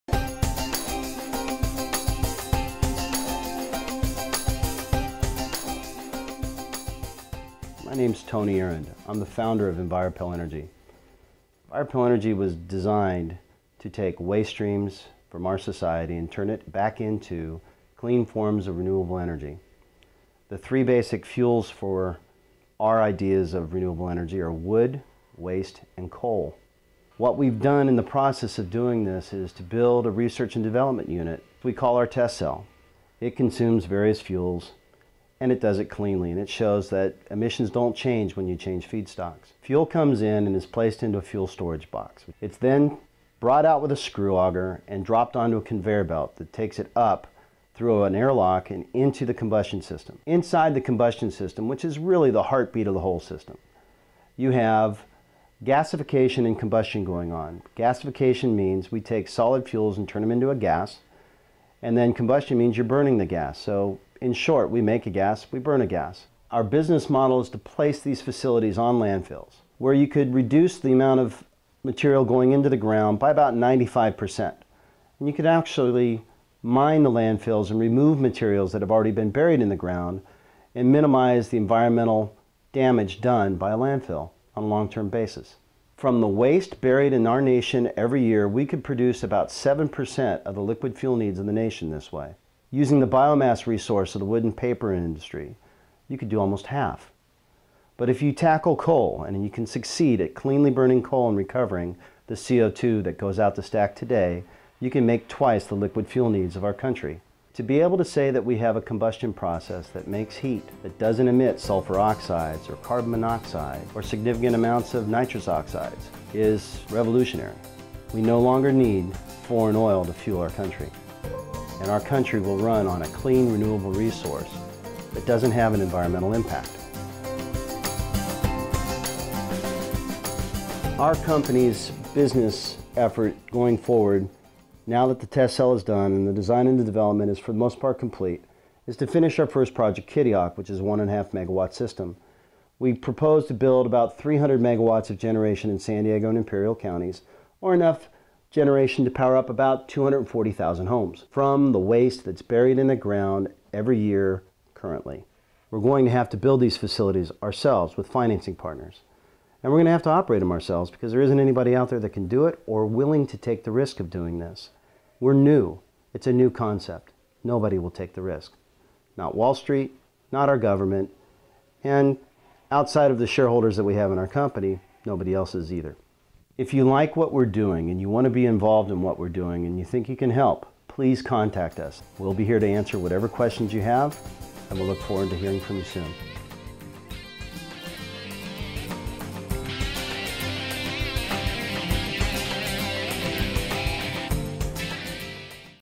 Envirepel Renewable Energy
Envirepel is a green energy production plant. Using a proprietary system for burning coal, wood, and trash (yes, I said trash!), the Envirepel furnace produces energy with close to zero emissions. This revolutionary technology has the potential to power hundreds of thousands of homes as an alternative fuel while simultaneously leaving no impact on the environment.

Unlike gas, electric, and solar power, the Envirepel system for burning fuel has been shown to be a reliable, low emissions, energy source for powering the nation and not a contributor to global warming or climate change (whatever your position on that issue may be). At the same time, Envirepel assists in reducing landfills by mining the trash piles for fuel!

The Envirepel system is fully operational and is currently producing energy but needs financial support to promote, educate, and export their system to local municipalities. Please support Envirepel by sharing this video on Facebook.

Envirepel is looking for investors.